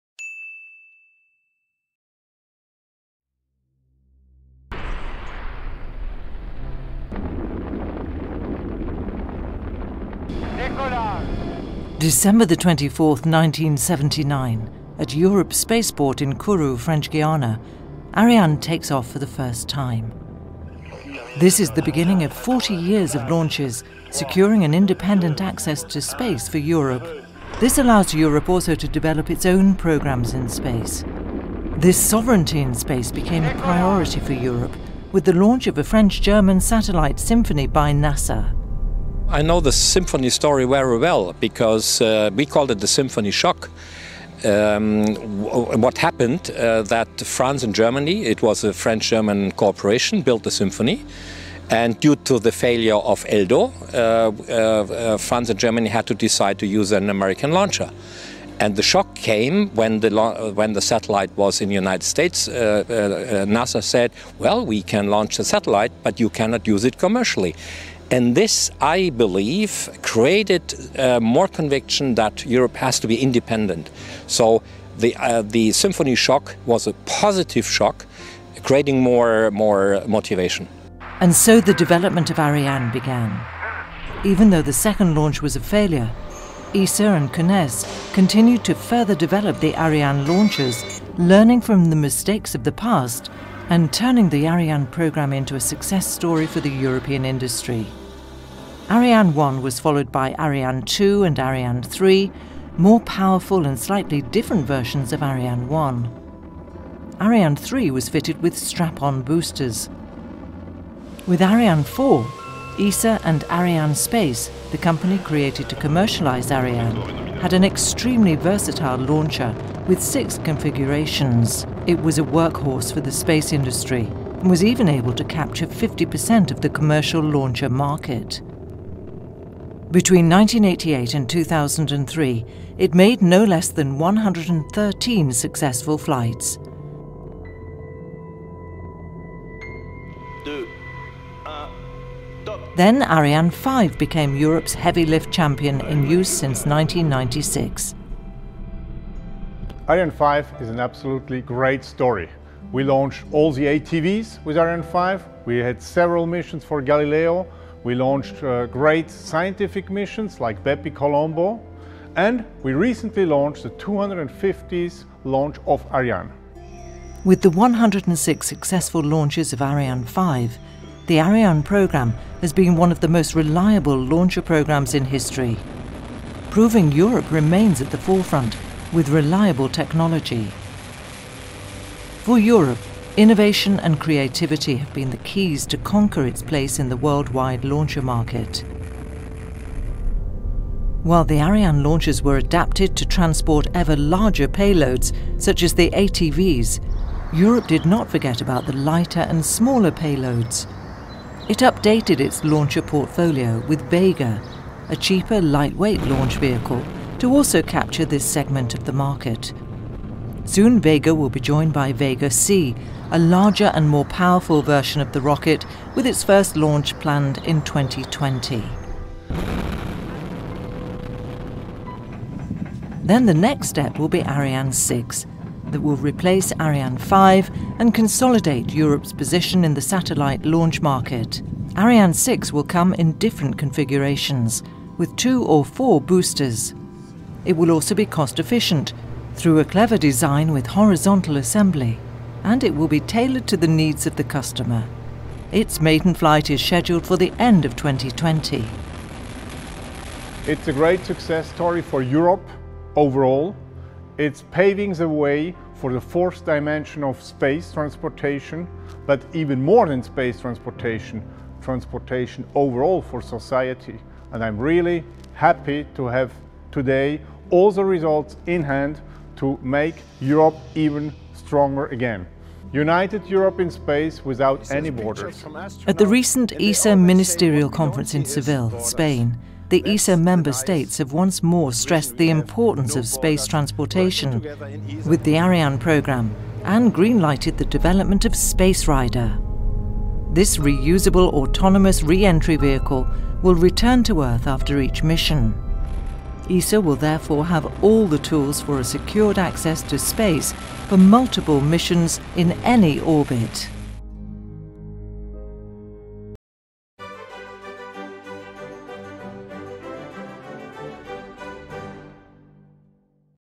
Ariane Launchers 40 Years of Innovative Development
The 250th flight of an Ariane launcher was another success. The Ariane launcher programme has effectively put Europe on the map in the worldwide commercial satellites launcher market. the 40th anniversary of the first Ariane Launch from Europe’ Spaceport in French Guiana and the situation has evolved along the years. An opportunity to look at what has been achieved and what can be expected for the future of Space Transportation in Europe.